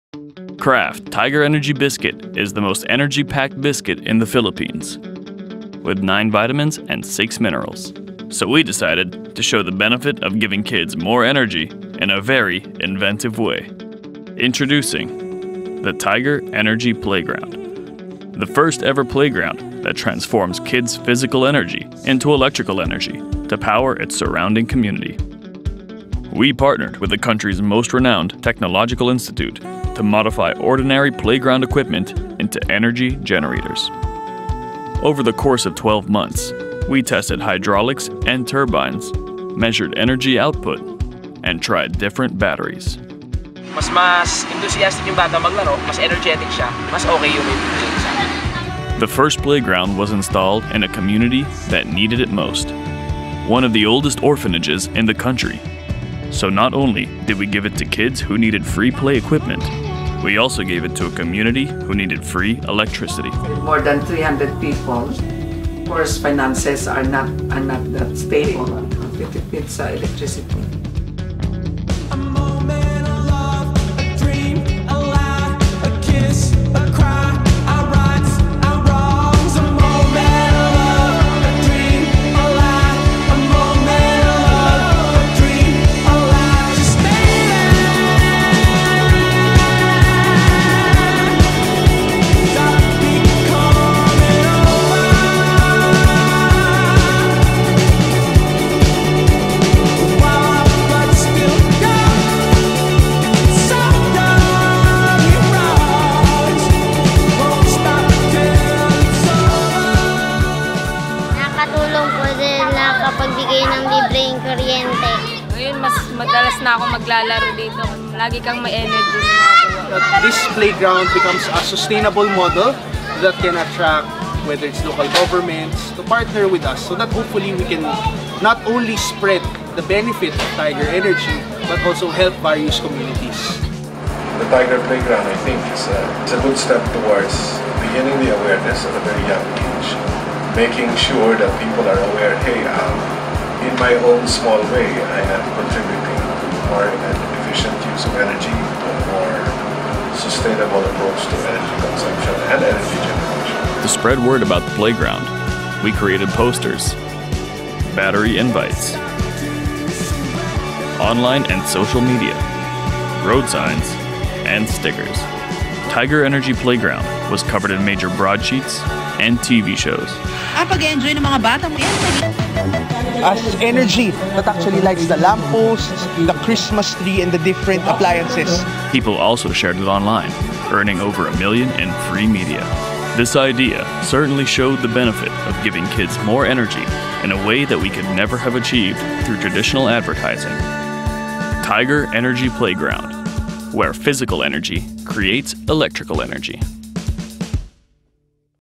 Energy Playground
Spikes Asia 2012 Silver Winner for Branded Content & Entertainment Category: Best Use or Integration of Experiential Events

Advertiser/Client: KRAFT PHILIPPINES
Product/Service: TIGER BISCUITS
Entrant Company: ACE SAATCHI & SAATCHI Manila, THE PHILIPPINES
Title: ENERGY PLAYGROUND
Contributing Company: ACE SAATCHI & SAATCHI Manila, THE PHILIPPINES
Executive Creative Director: Andrew Petch
Creative Director: Greg Martin
Copywriter: Tony Sarmiento/Abe Medenilla/Paolo Agulto/Petra Magno/Maui Reyes
Art Director: Gelo Lico/Milton Ganelo/Glenn Lalogan
Account Supervisor: Maine Gatbonton/Gigi Garcia/Rose Javier
Activation Head: Gen Lizares/Onat Roldan
Digital Director: Shiela Tiongco
Events Manager: Louie Di Jr
Events Director: Kiko Torno/Kris Cortez
PR: Martin Romualdez
Activations Communications Director: Patti Mallari
Digital Manager: Jerik De Guzman
Final Artist: Gelo Lico/Rico Torres
Producer: Carlo Perlas
Photographer: Paolo Ruiz